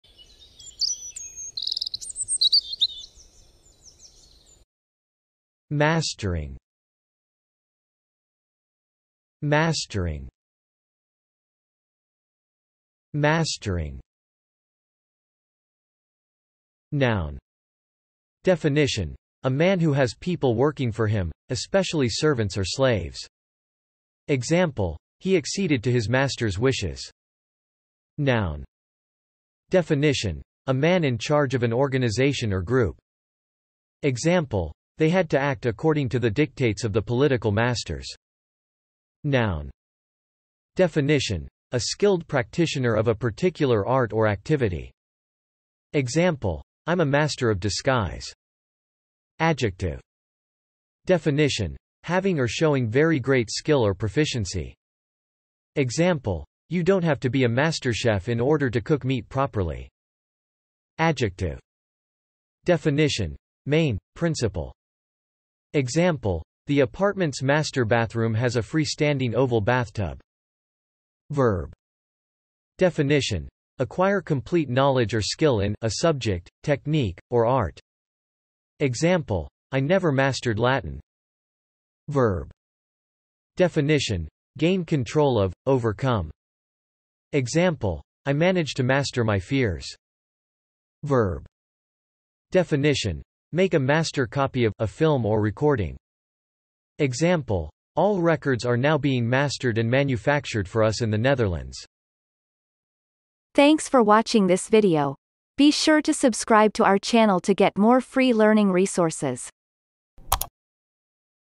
How to pronounce mastering - Vocab Today
The pronunciation of the word mastering in Amercian accent is demonstrated in this video. If the word is from another language, such as brand name, it will be pronounced in its original language. Based on the word frequency, this is of 3 stars difficulty.

The word mastering has the following meanings:
1. (noun)
Definition : A man who has people working for him, especially servants or slaves.
Example : he acceded to his master's wishes
2. (noun)
Definition : A man in charge of an organization or group.
Example : They had to act according to the dictates of the political masters.
3. (noun)
Definition : A skilled practitioner of a particular art or activity.
Example : I'm a master of disguise
4. (adjective)
Definition : Having or showing very great skill or proficiency.
Example : you don't have to be a master chef in order to cook meat properly
5. (adjective)
Definition : Main; principal.
Example : the apartment's master bathroom has a free-standing oval bathtub
6. (verb)
Definition : Acquire complete knowledge or skill in (a subject, technique, or art)
Example : I never mastered Latin
7. (verb)
Definition : Gain control of; overcome.
Example : I managed to master my fears
8. (verb)
Definition : Make a master copy of (a film or recording)
Example : all records are now being mastered and manufactured for us in the Netherlands